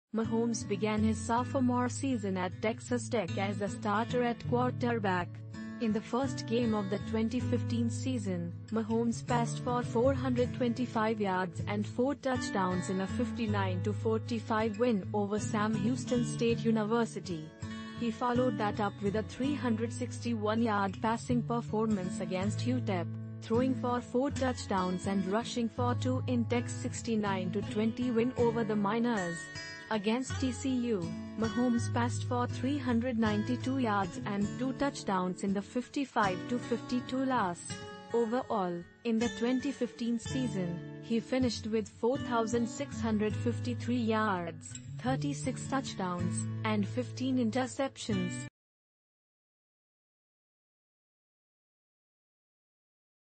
Patrick Mahomes - Sophomore
Mahomes began his sophomore season at Texas Tech as the starter at quarterback. In the first game of the 2015 season, Mahomes passed for 425 yards and four touchdowns in a 59–45 win over Sam Houston State University. He followed that up with a 361-yard passing performance against UTEP, throwing for four touchdowns and rushing for two in Tech's 69–20 win over the Miners. Against TCU, Mahomes passed for 392 yards and two touchdowns in the 55–52 loss . Overall, in the 2015 season, he finished with 4,653 yards, 36 touchdowns, and 15 interceptions.